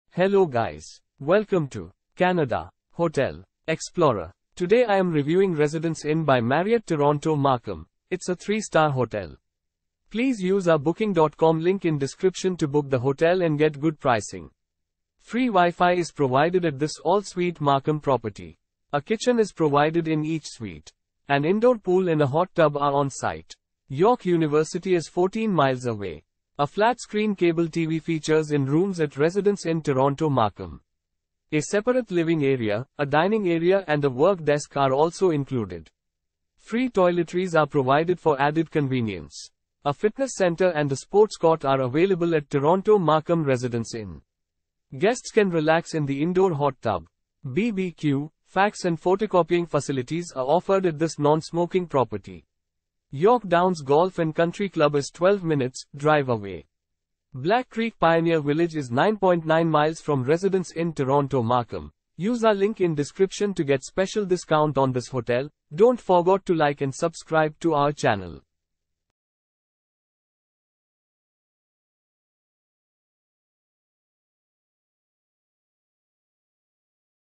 Residence Inn by Marriott Toronto Markham Review - Markham , Canada
Residence Inn by Marriott Toronto Markham

Free Wi-Fi is provided at this all-suite Markham property. A kitchen is provided in each suite. An indoor pool and a hot tub are on site. York University is 14 mi away.

A flat-screen cable TV features in rooms at Residence Inn Toronto Markham. A separate living area, a dining area and a work desk are also included. Free toiletries are provided for added convenience.

A fitness center and a sports court are available at Toronto Markham Residence Inn. Guests can relax in the indoor hot tub. BBQ, fax and photocopying facilities are offered at this nonsmoking property.

York Downs Golf and Country Club is 12 minutes’ drive away. Black Creek Pioneer Village is 9.9 mi from Residence Inn Toronto Markham.